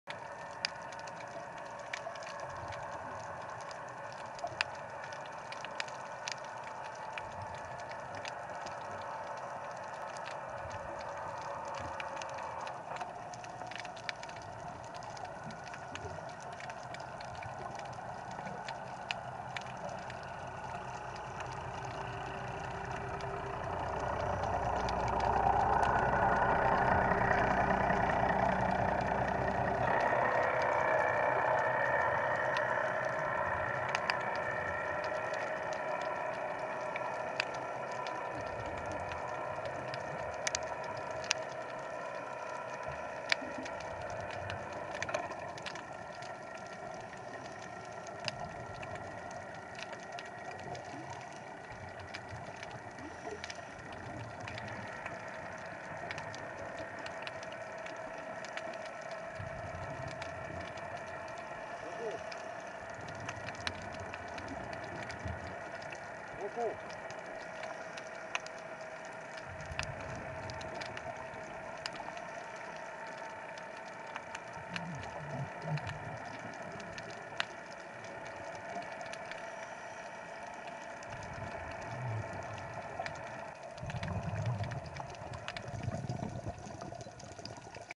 aproximación peligrosa 2018 09 20
Buceo en el Bajo de Fuera de hoy. Una embarcación pasa exactamente por encima de mí con la hélice a toda pastilla cuando estoy a tres metros preparándome para la salida.